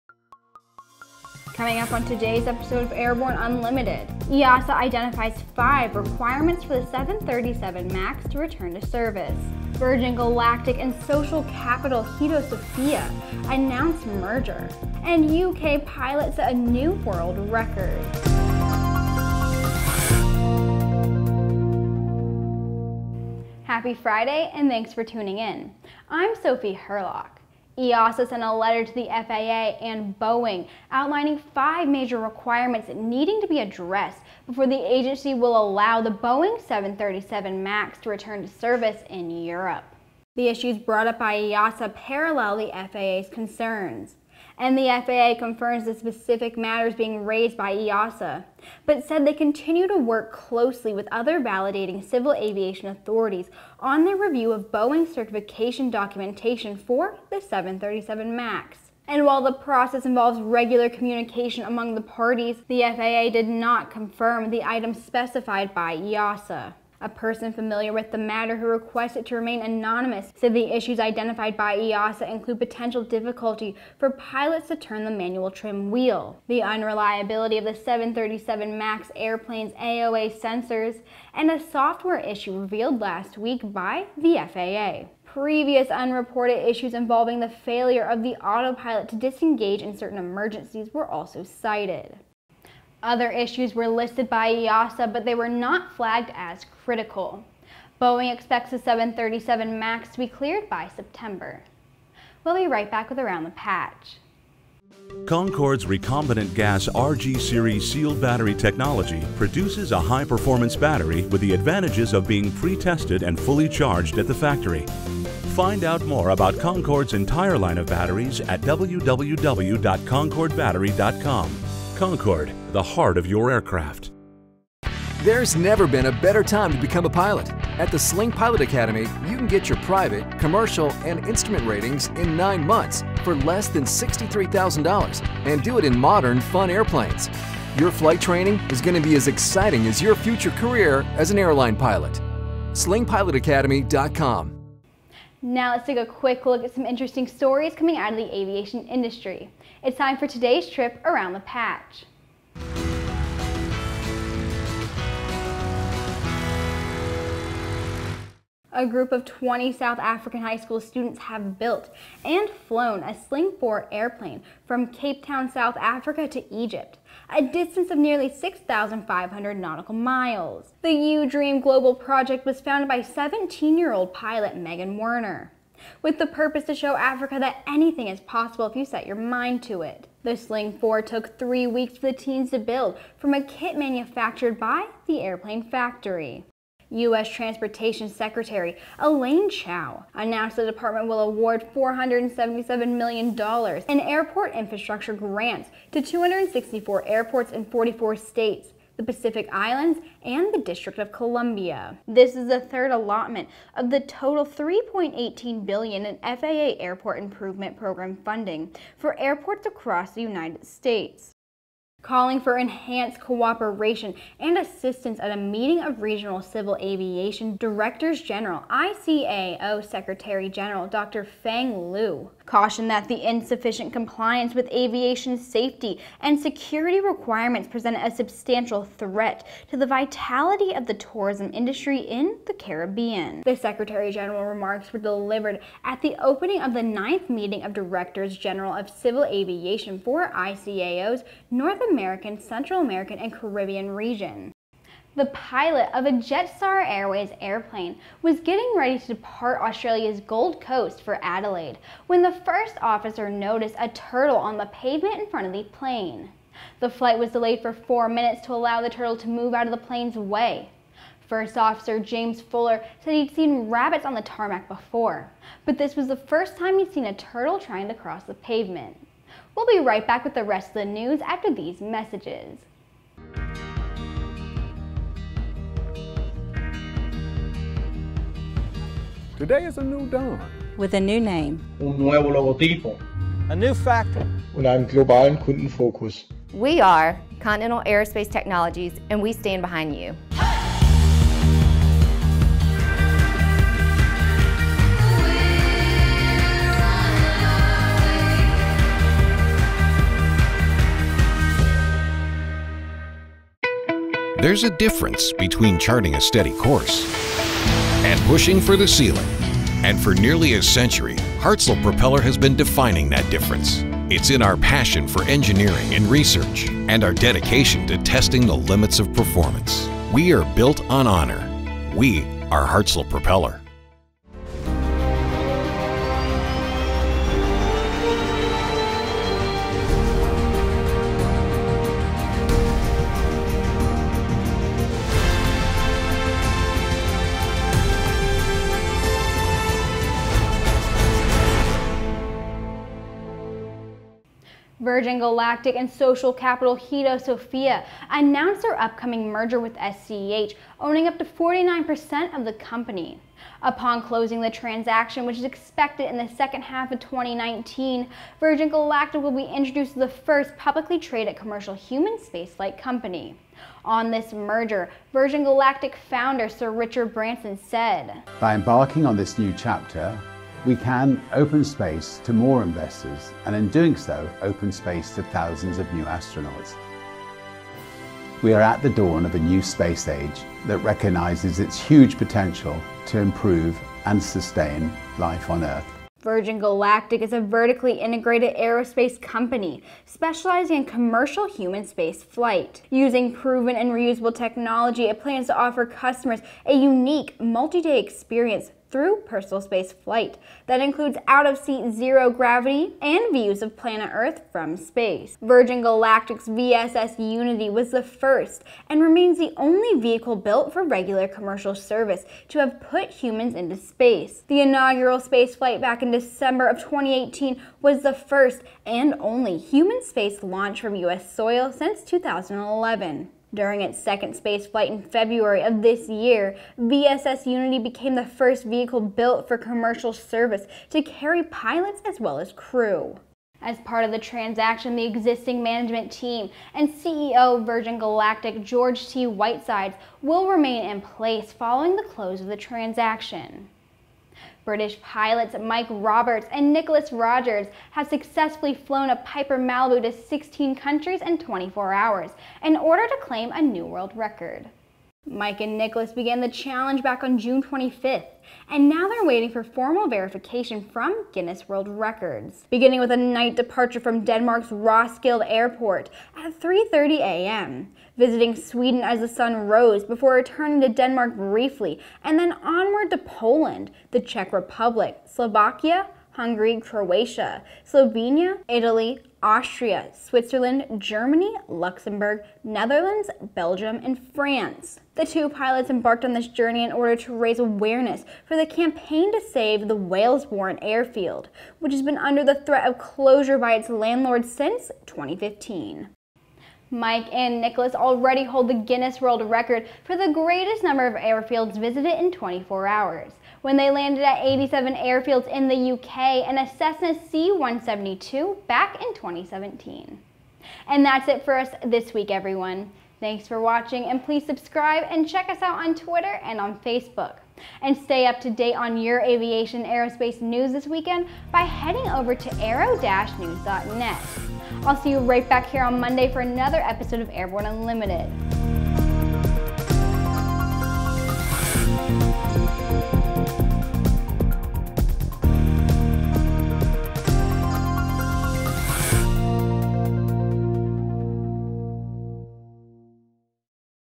Airborne 07.12.19: 737 MAX RTS?, Virgin Galactic Merges, UK Pilot Record
Also: Teenagers Fly Sling 4, Infrastructure Grants, Turtle On The Tarmac, Focus On Aviation Safety

EASA has sent a letter to the FAA and Boeing outlining five major requirements the agency says need to be addressed before it will allow the Boeing 737 MAX to return to service in Europe. The issues identified by EASA parallel the FAA's concerns. The FAA said that the agency "confirm(s) the specific matters being raised by EASA but said: “The FAA continues to work closely with other validating civil aviation authorities on our review of Boeing’s certification documentation for the 737 MAX. This process involves regular communications among all parties" but did not confirm the items specified by EASA. Virgin Galactic and Social Capital Hedosophia, have announced their upcoming merger, with SCH owning up to approximately 49% of the company. Upon closing of the transaction, which is expected in the second half of 2019, VG will be introduced as the first and only publicly traded commercial human spaceflight company. VG is a vertically integrated aerospace company specializing in commercial human spaceflight. Using proven and reusable technology, it intends to offer its customers a unique, multi-day experience culminating in a personal spaceflight that includes out-of-seat zero gravity and views of Planet Earth from space. British pilots Mike Roberts and Nicholas Rogers have successfully flown a Piper Malibu to 16 countries in 24 hours to claim a new World Record. Mike, owner of flying club and flight training school Take Flight Aviation, based at Wellesbourne Airfield, Warwickshire and Nicholas Rogers, a B737 Captain based at Birmingham Airport, undertook the challenge on Tuesday, June 25th. They now await formal verification of their feat from Guinness, which currently lists the most countries visited by a fixed wing aircraft in 24 hrs as 11. Mike and Nicholas embarked on the initiative to raise awareness of the ongoing campaign to Save Wellesbourne Airfield, which since 2015 has been under threat from closure from its Landlords. All this -- and MORE in today's episode of Airborne!!!

Airborne 07.12.19 is chock full of info in this Daily News Episode, Friday, July 12th, 2019... Presented by Aero-TV veteran videographer and Airborne Host Sophie Hurlock and is supported by ANN CEO/Editor-In-Chief Jim Campbell, Chief Videographer Nathan Cremisino, and Aero-Journalist Tom Patton.